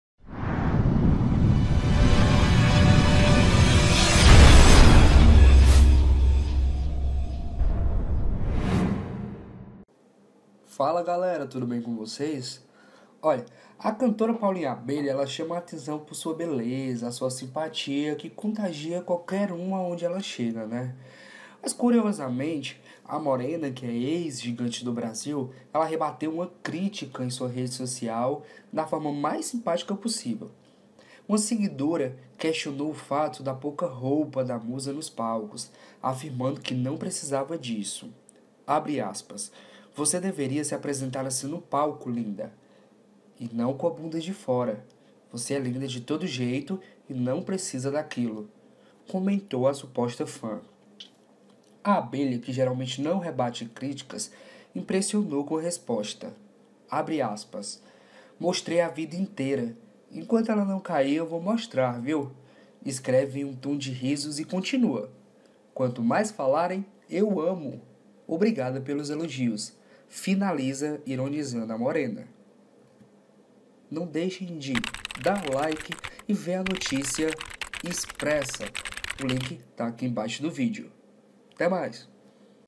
Seguidora questiona bumbum de Paulinha Abelha e cantora rebate: " Enquanto não cair vou mostrar "
Facebook Grupo : Portal Cearabelha
Facebook Página : Portal Cearabelha
Periscope : Cearabelha
Google+ : Cearabelha